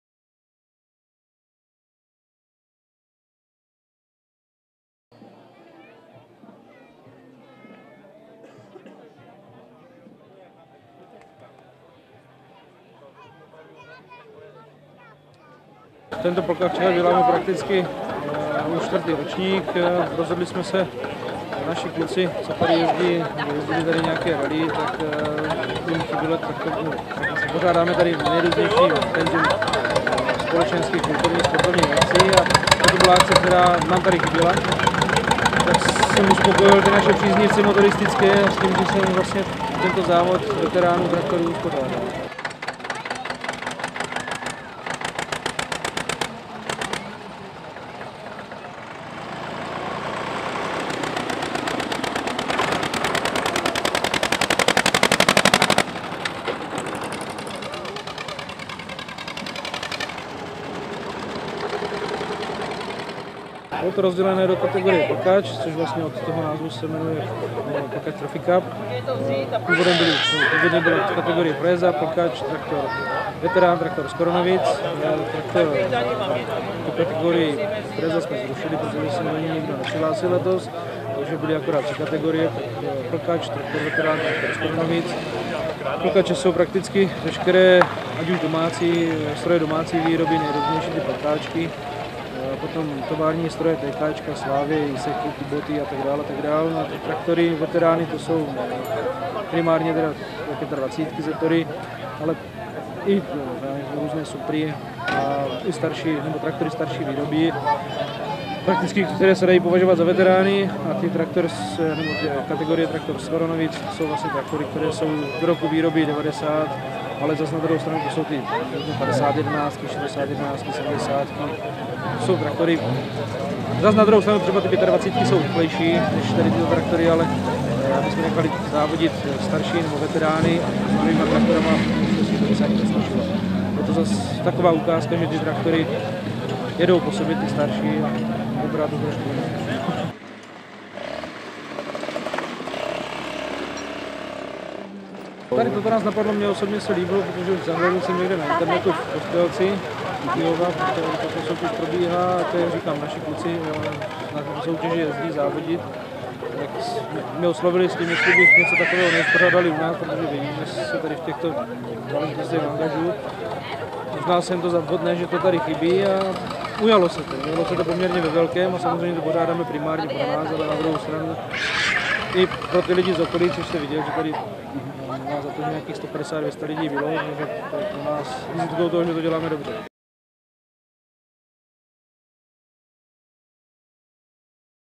Josefov Plkač Trophy Cup
V Josefově se konal čtvrtý ročník „Plkač Trophy Cup“ závod fréz, plkačů, traktorů veteránů a traktorů skoronoviců. Akce se pod záštitou obce vydařila, nechybělo mnoho doprovodných soutěží jak pro závodníky tak i děti.